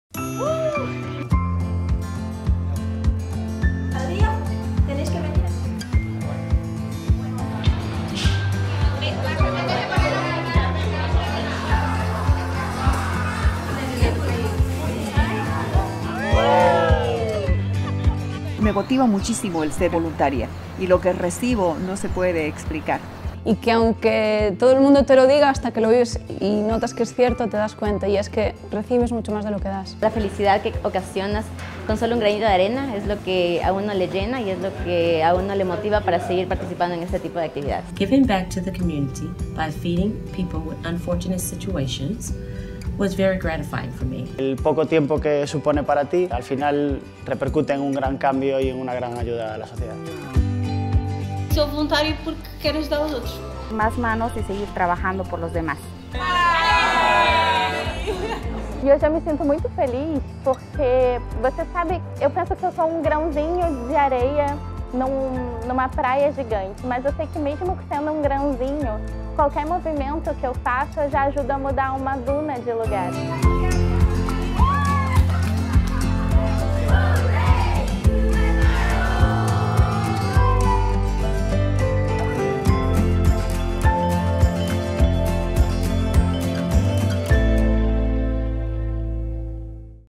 Repsol International Volunteering Week – Fundación Repsol
About a thousand Repsol volunteers from 22 countries working for the SDGs: Zero Hunger and Quality Education. Repsol International Volunteering Week has contributed to improve the quality of life of more than 12,000 people, with the collaboration of 70 social entities. Thank you all for joining the social energy!